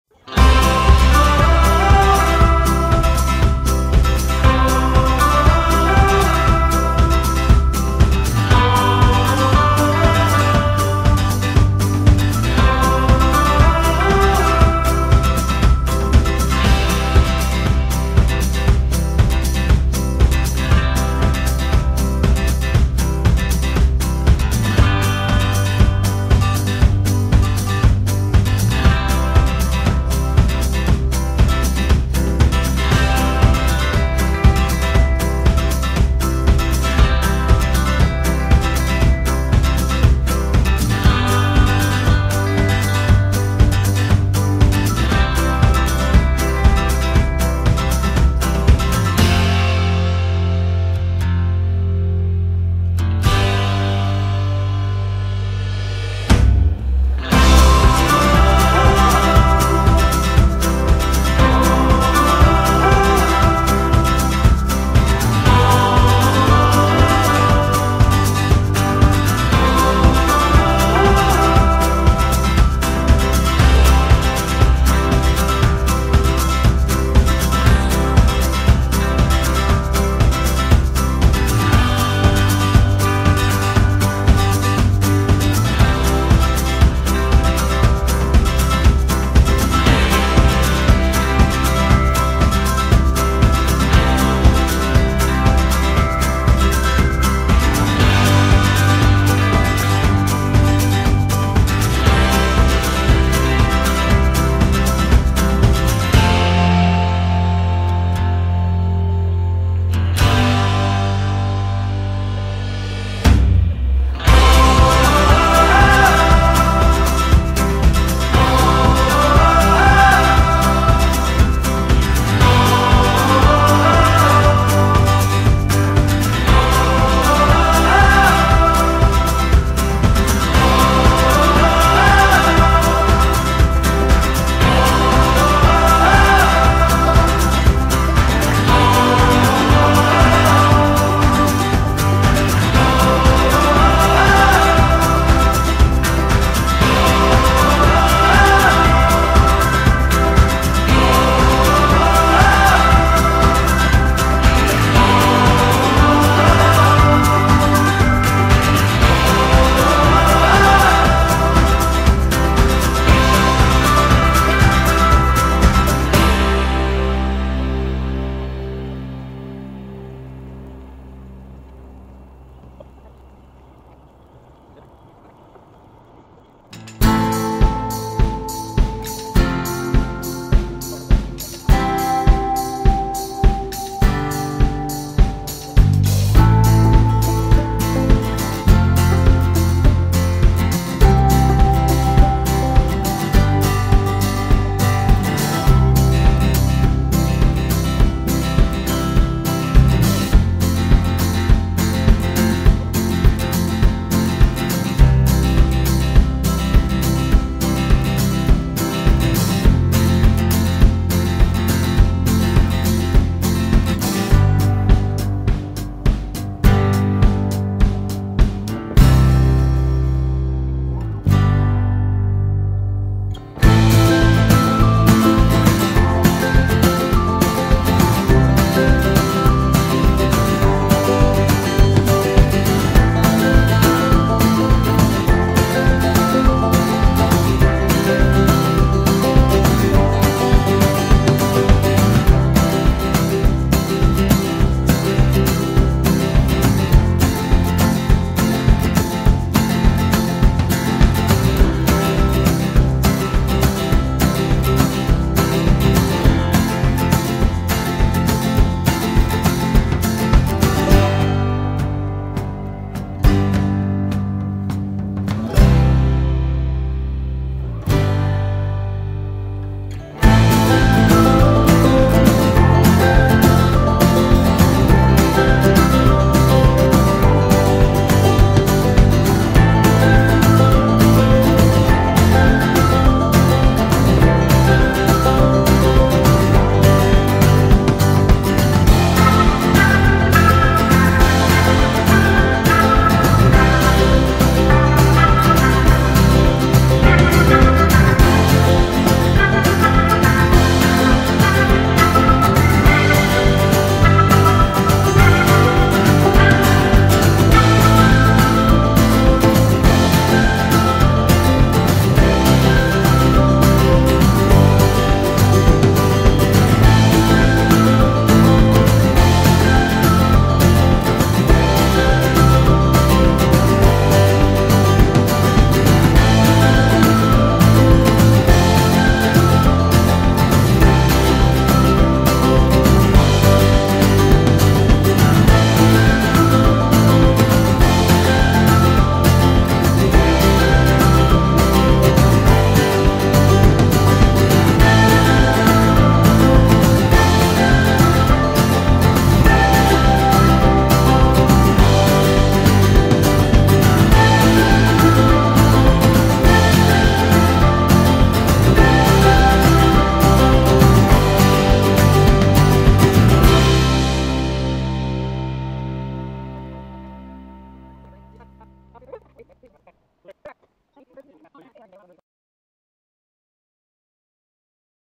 Hampton Beach Time Lapse
Westfield, MA to Hampton Beach, NH in 6 minutes